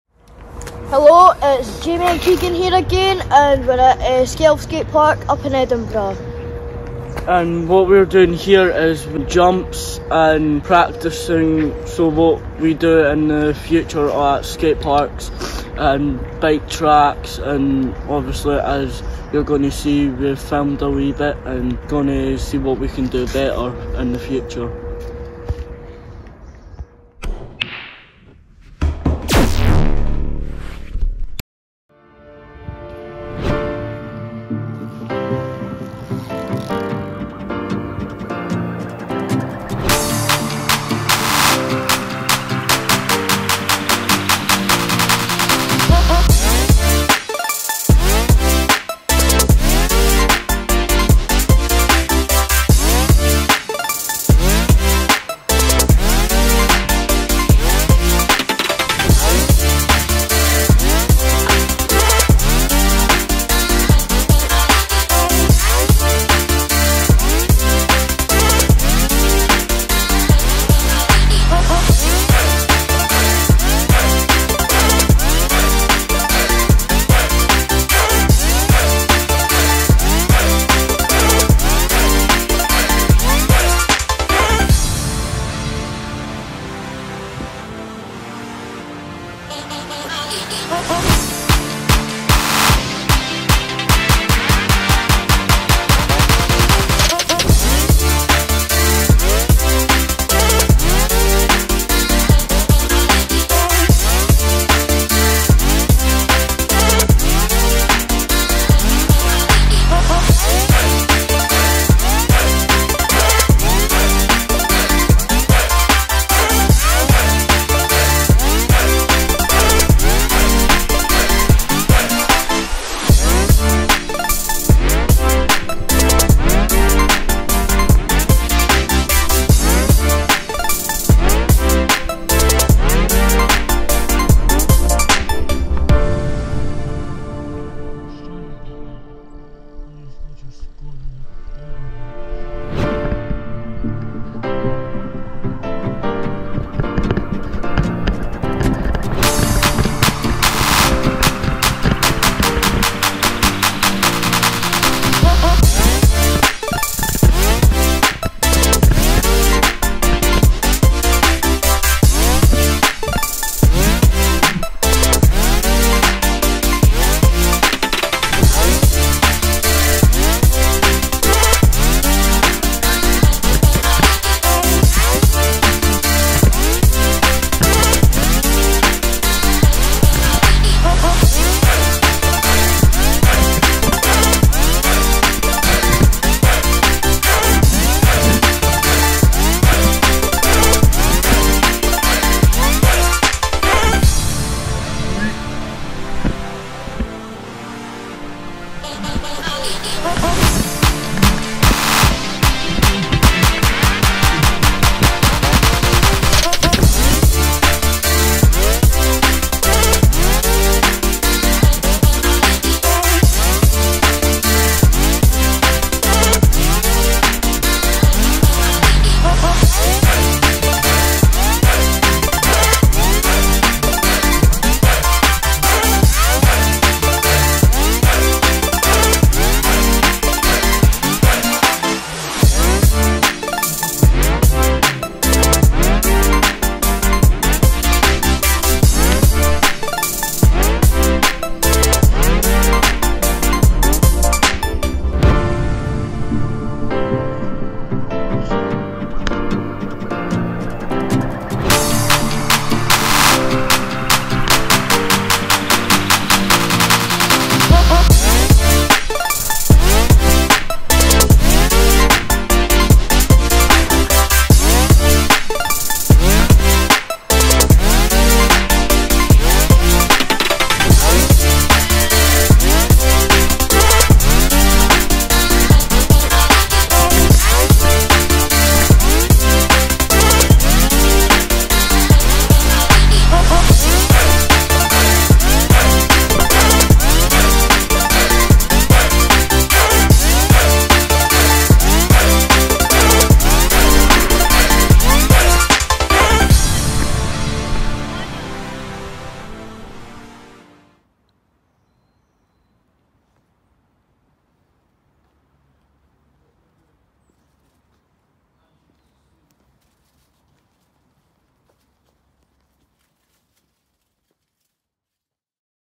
Keigan & Jamie's Adventures - Episode 3
In this weeks' episode the boys are ripping through SKELF pumptrack in Edinburgh